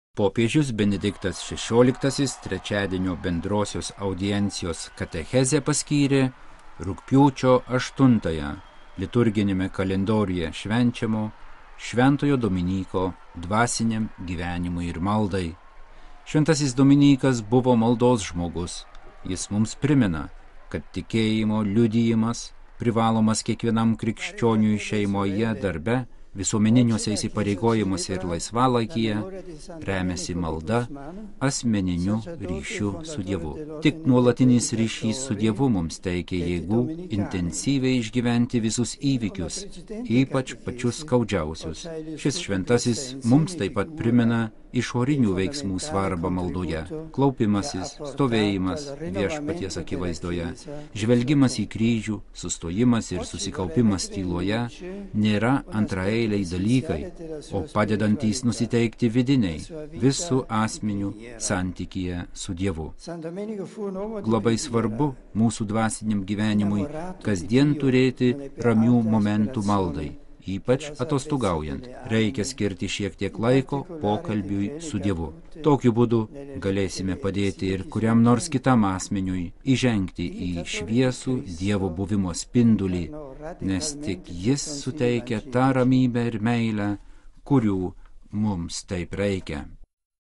Benediktas XVI: Sekti šv. Dominyką maldoje
Popiežius Benediktas XVI trečiadienio bendrosios audiencijos katechezę rugpjūčio 8-ąją paskyrė liturginiame kalendoriuje švenčiamo šv. Dominyko dvasiniam gyvenimui ir maldai. Šv. Dominykas mums primena, kad tikėjimo liudijimas, privalomas kiekvienam krikščioniui šeimoje, darbe, visuomeniniuose įsipareigojimuose ir laisvalaikyje, remiasi malda, asmeniniu ryšiu su Dievu. Tik nuolatinis ryšys su Dievu mums teikia jėgų intensyviai išgyventi visus įvykius, ypač pačius skaudžiausius. Šis šventasis mums taip pat primena išorinių veiksmų svarbą maldoje: klaupimasis, stovėjimas Viešpaties akivaizdoje, žvelgimas į Kryžių, sustojimas ir susikaupimas tyloje nėra antraeiliai dalykai, o padedantys nusiteikti vidine ...